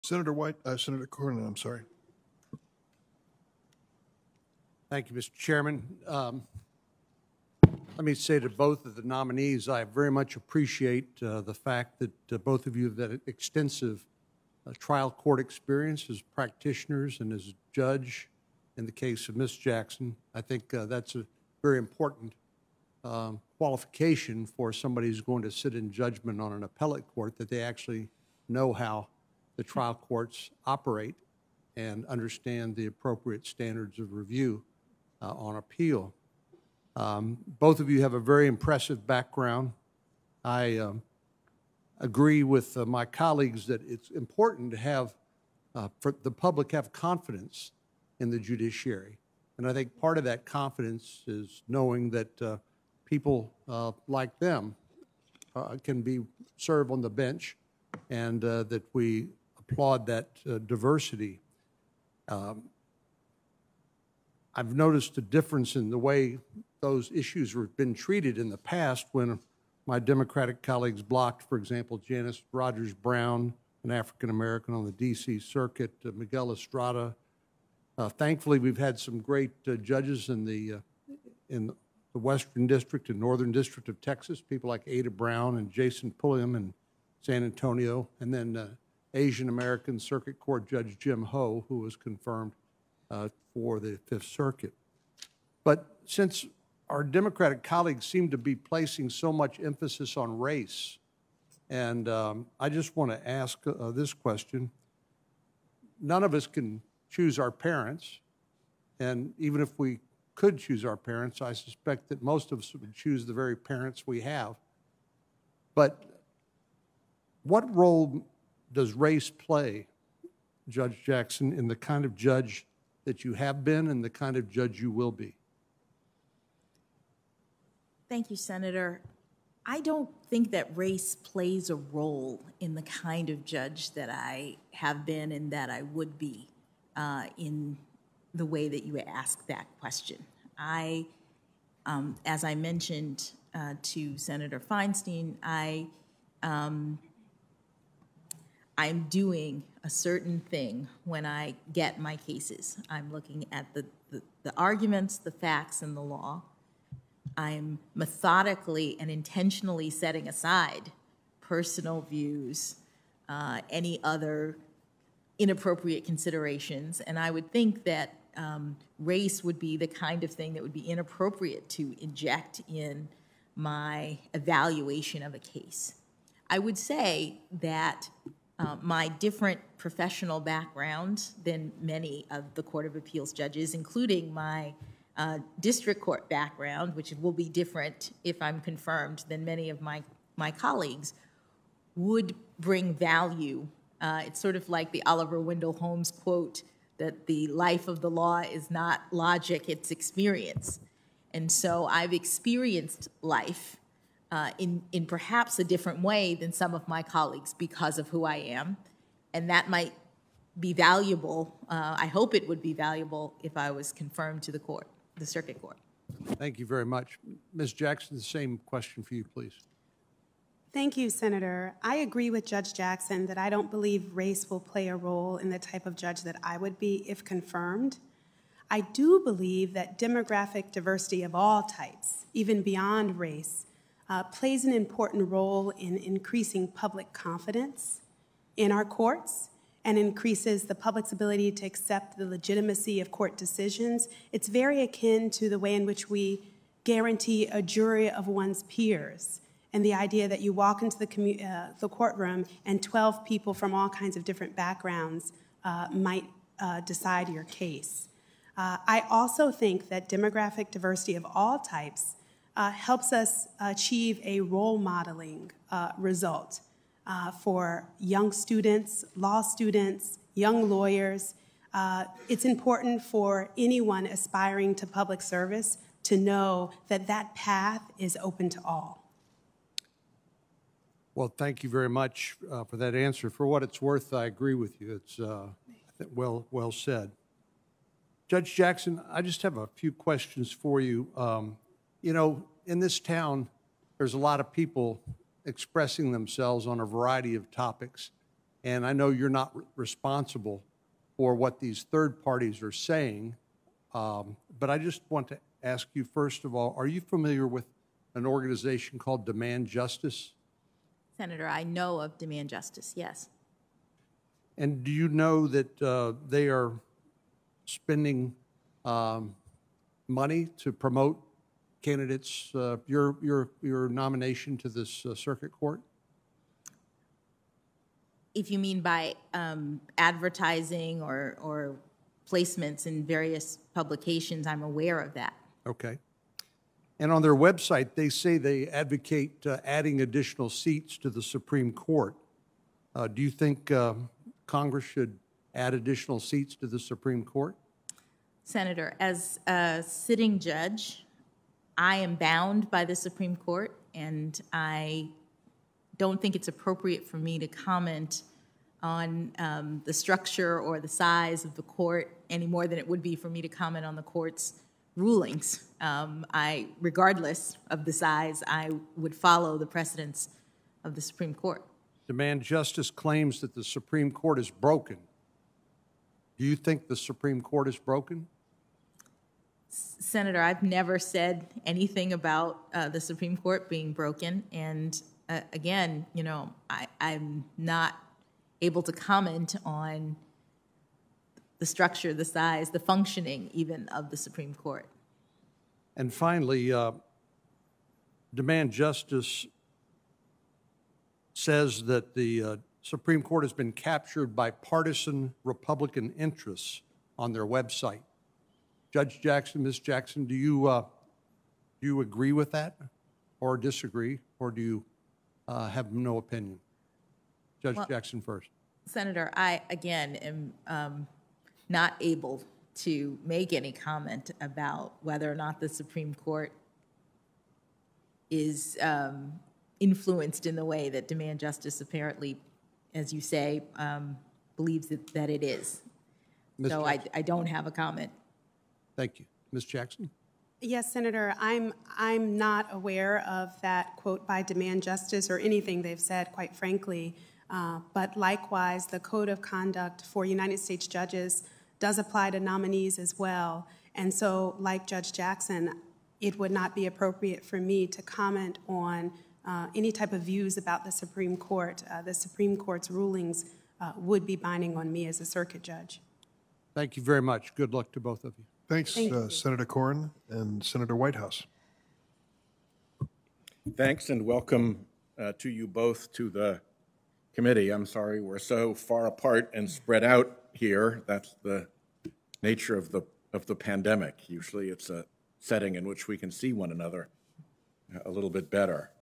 GOP Senator: "Our Democrat Colleagues Seem To Be Placing So Much Emphasis On Race"
At Wednesday's Senate Judiciary confirmation hearing, Sen. John Cornyn accused Democrats of "placing so much emphasis on race" and asked judicial nominees Judge Ketanji Brown Jackson and Candace Jackson-Akiwumi "what role does race play" in the kind of Circuit Judges they'd be.